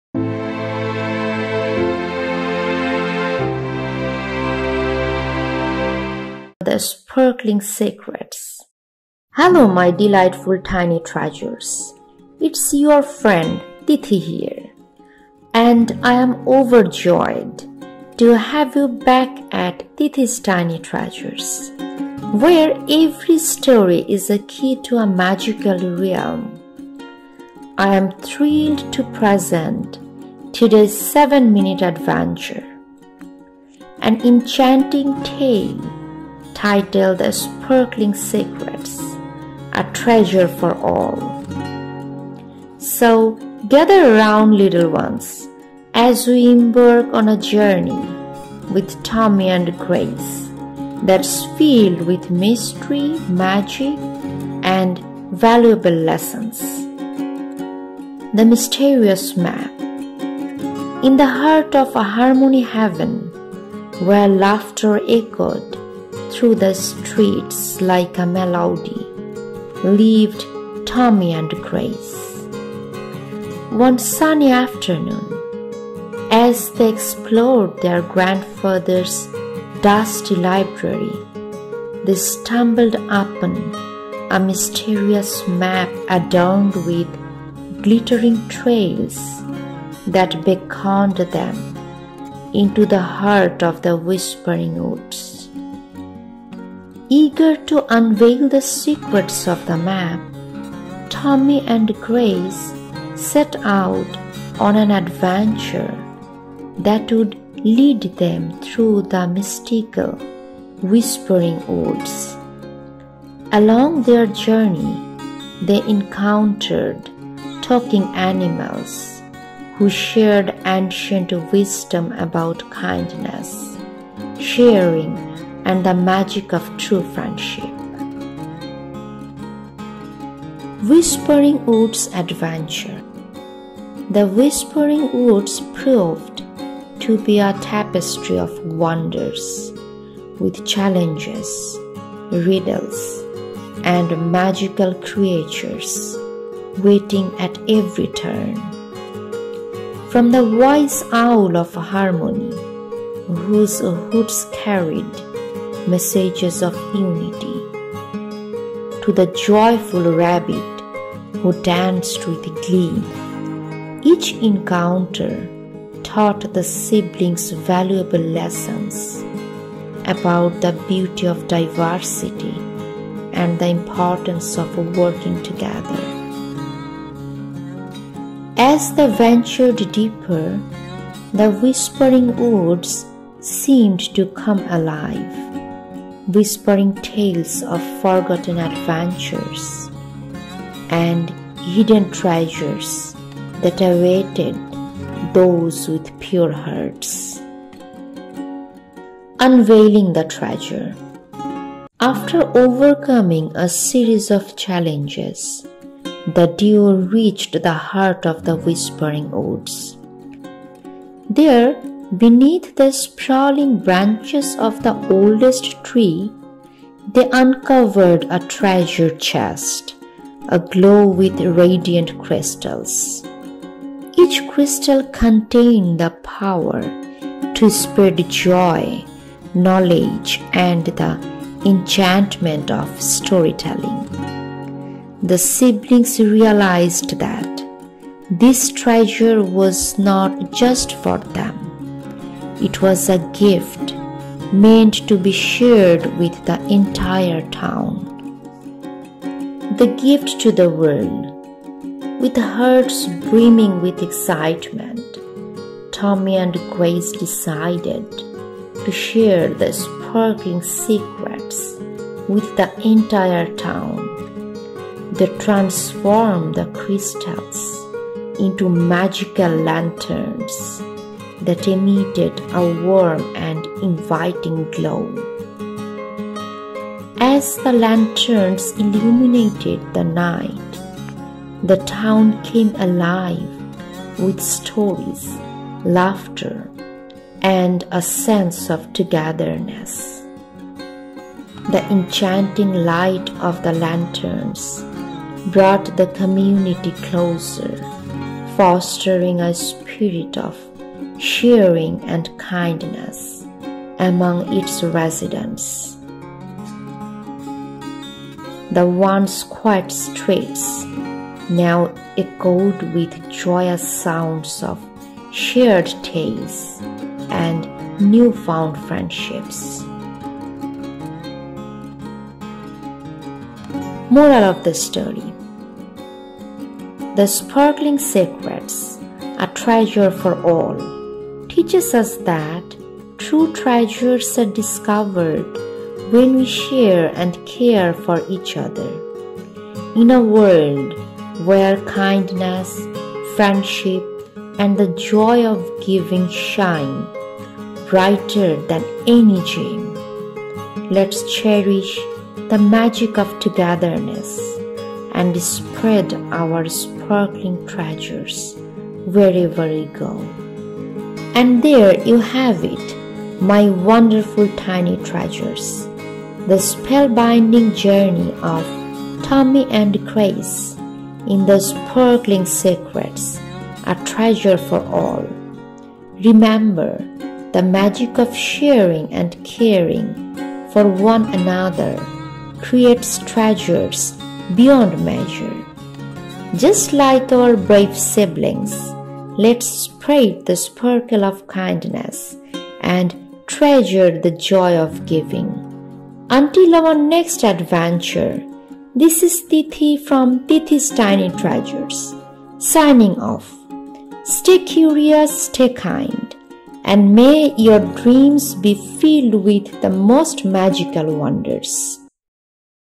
The Radiant Crystals: Sparkling Secrets for Tiny Treasures
🌟 Welcome to Tethi's Tiny Treasures! 🌟
📚 Join us in today's enchanting adventure, "The Sparkling Secrets: A Treasure for All." In this magical tale, siblings Tommy and Grace embark on a whimsical journey through the Whispering Woods, discovering the true meaning of friendship, kindness, and the joy of giving.

🍃 Part 1: Explore the dusty library with Tommy and Grace as they stumble upon a mysterious map that unveils glittering trails leading to an enchanted world.

🌲 Part 2: Venture into the Whispering Woods, where talking animals share ancient wisdom, and the forest comes alive with the magic of diversity and cooperation.

💎 Part 3: Witness the siblings unveil a chest filled with radiant crystals, each holding the power to spread joy, knowledge, and the enchantment of storytelling.

🌟 Part 4: Tommy and Grace decide to share their magical discovery with the entire town, transforming crystals into lanterns that light up the night with stories, laughter, and newfound friendships.

🎉 Outro: As we wrap up this magical journey, Tethi from Tethi's Tiny Treasures encourages us to spread the sparkle of kindness and treasure the joy of giving.

🌈 Moral of the Story: "The Sparkling Secrets: A Treasure for All" reminds us that true treasures are discovered when we share and care for each other. Let's cherish the magic of togetherness and spread our own sparkling treasures wherever we go.